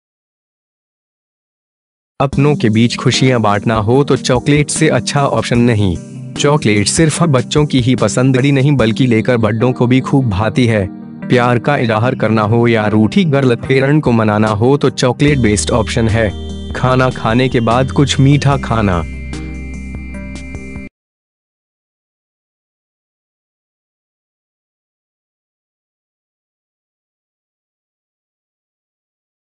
चॉकलेट में है सेहतभरे फायदे
अपनों के बीच खुशियां बांटना हो तो चॉकलेट से अच्छा ऑप्शन नहीं। चॉकलेट सिर्फ बच्चों की ही पसंददी नहीं बल्कि लेकर बडों को भी खूब भाती है। प्यार का इजाहर करना हो या रूठी गर्लफे्रंड को मनाना हो तो चॉकलेट बेस्ट ऑप्शन है। खाना खाने के बाद कुछ मीठा खाना...